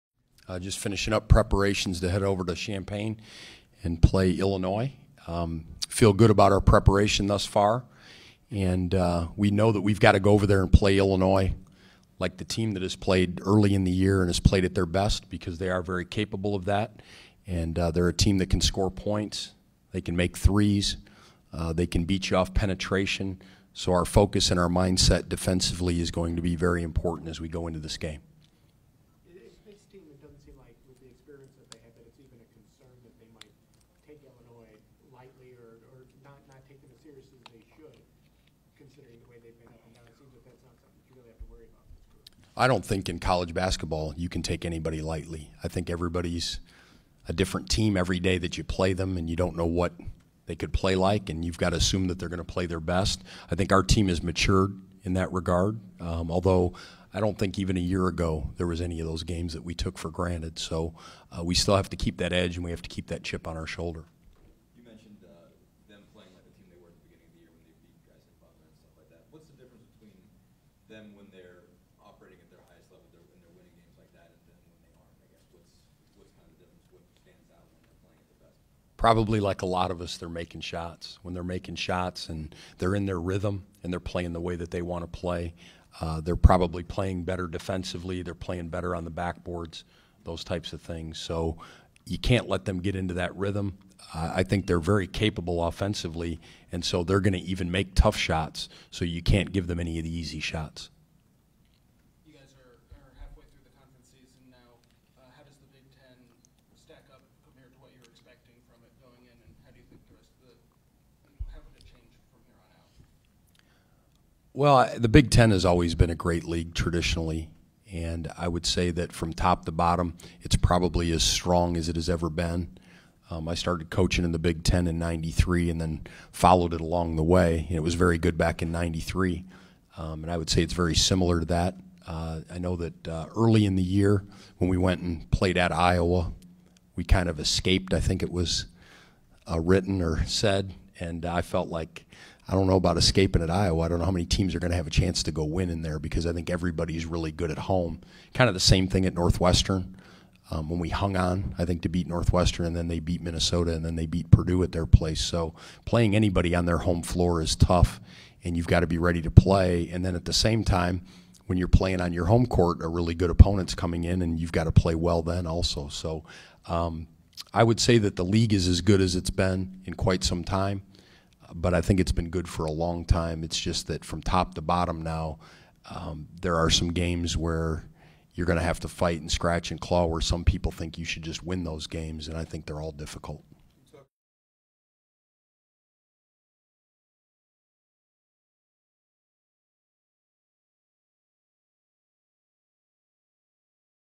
Tim Buckley Press Conference - Feb. 6, 2013
Indiana associate head men's basketball coach Tim Buckley previews the Hoosiers' game at Illinois on Thursday, Feb. 7.